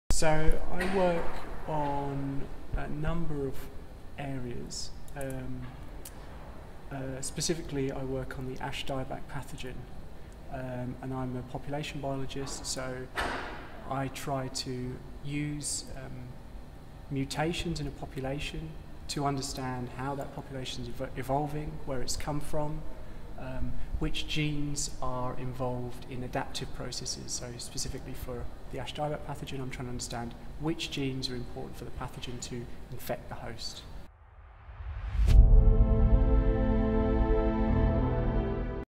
Population Genomics with Dr Mark McMullan
We ask Mark what the aim of his population genomics research is.